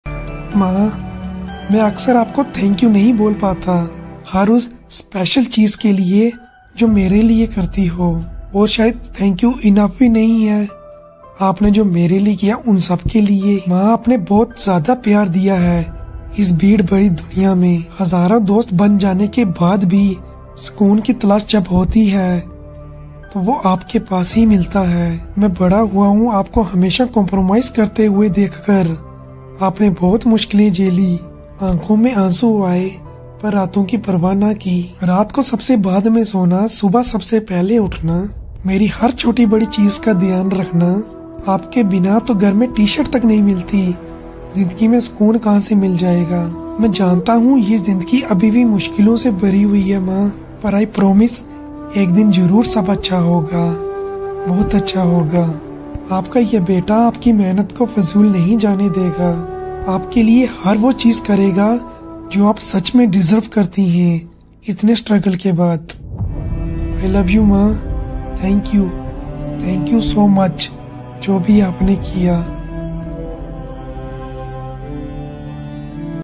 Thank You Maa! ❤️ | Poetry For Maa | Hindi Lines On Mother | Mother's day | By Gourav Sharma
Video Direction - Anuj Sharma
Gourav Sharma
Video editor -Anuj sharma || Gourav Sharma

hit likes.